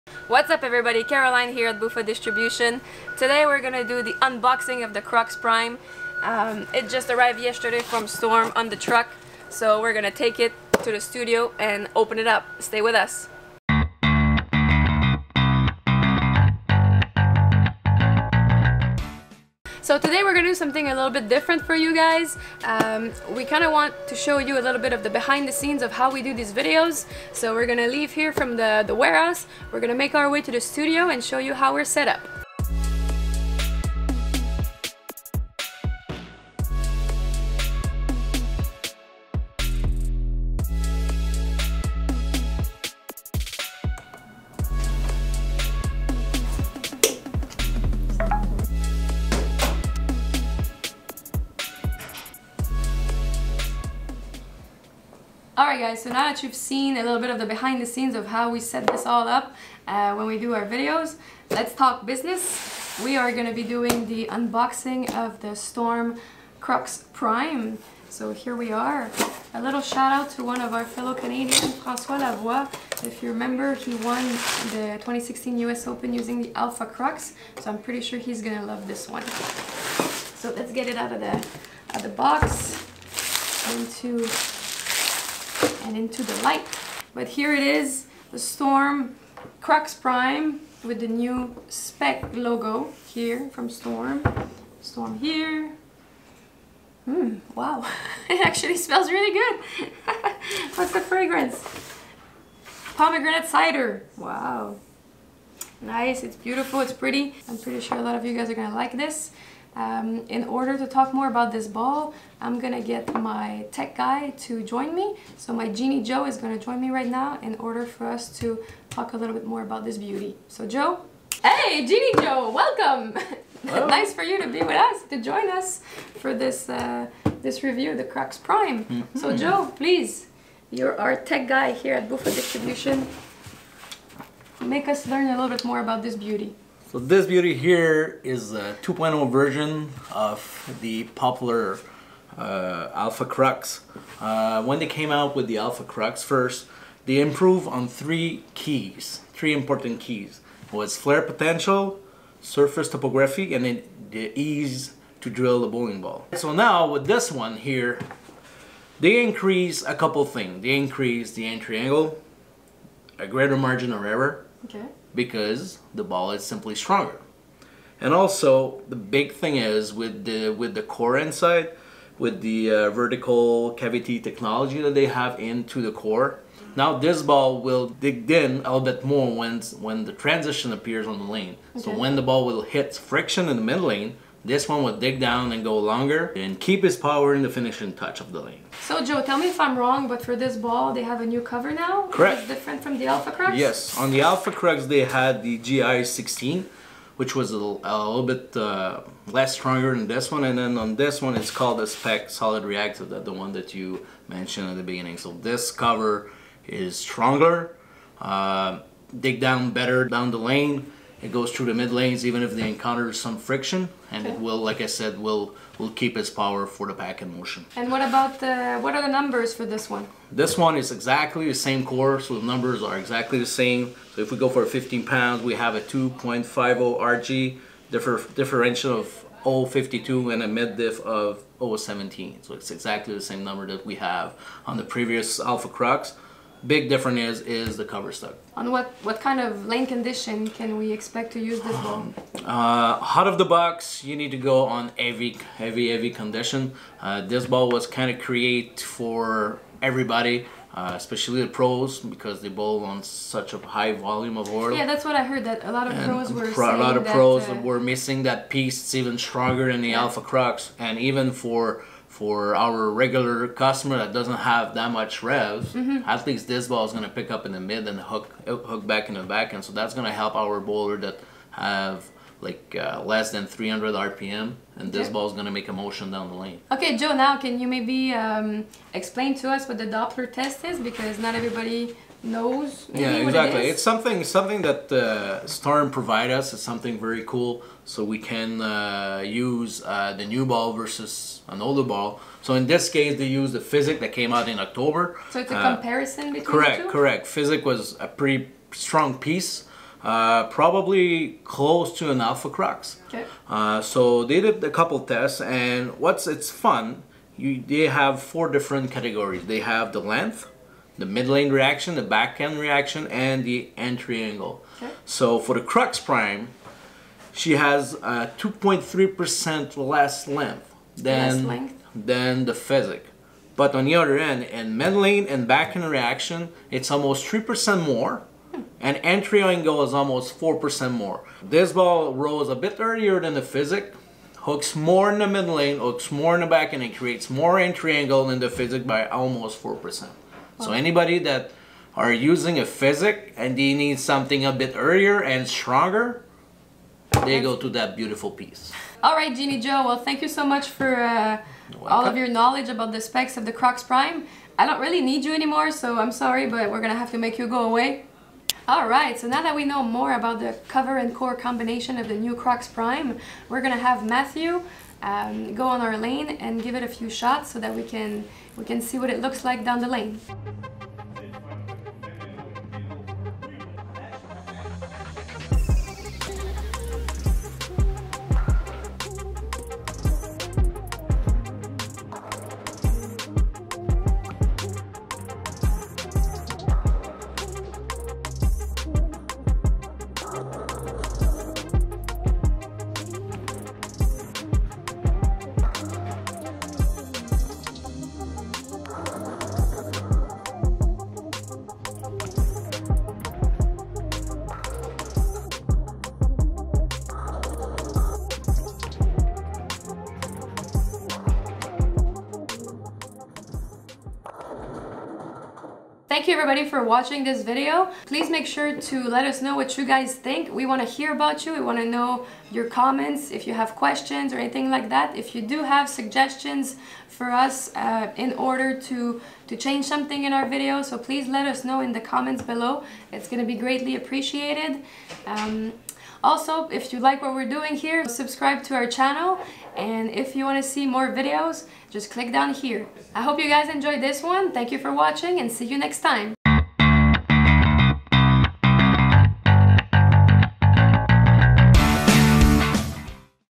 Storm Crux Prime | Unboxing & Review
Caroline Lagrange & Jonathan Simoneau are unboxing the New Crux Prime from Storm Bowling. After that, Mathieu Ménard is going to throw the Crux Prime on a fresh Abbey Road (Kegel Challenge Series, 40').

Product featured in this video